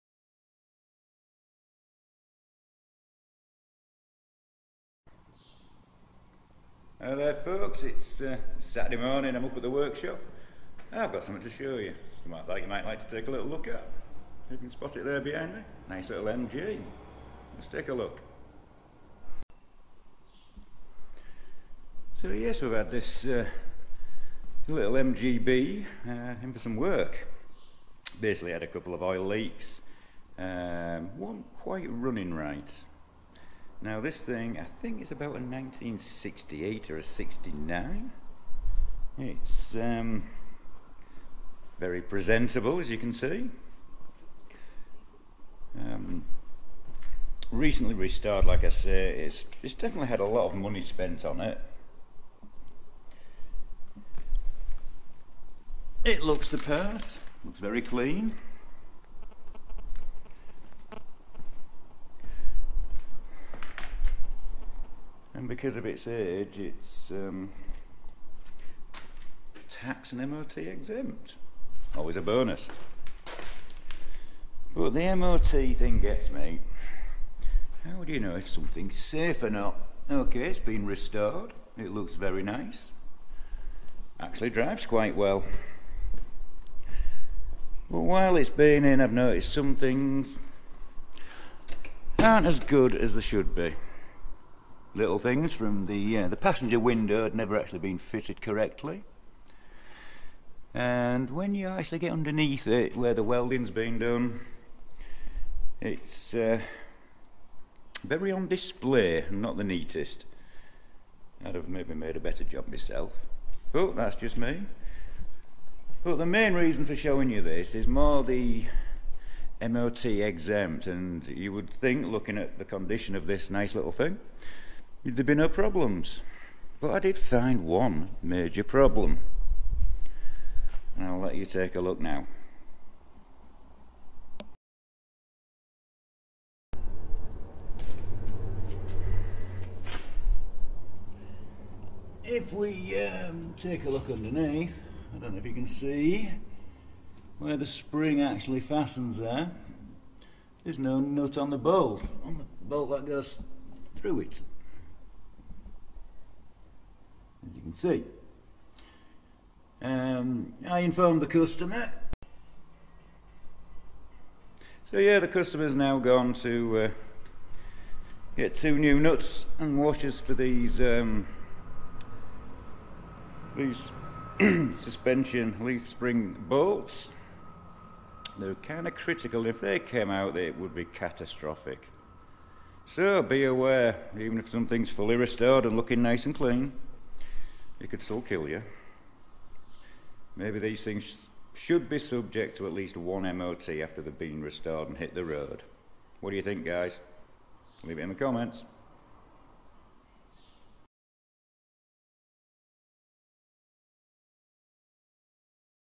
The DANGERS Of MOT Exemption ....
Had this delightful little MGB in the workshop this week, came in for an oil leak and bad running. which was easily sorted. however something I spotted on this fully restored MOT exempt car couldn't be ignored.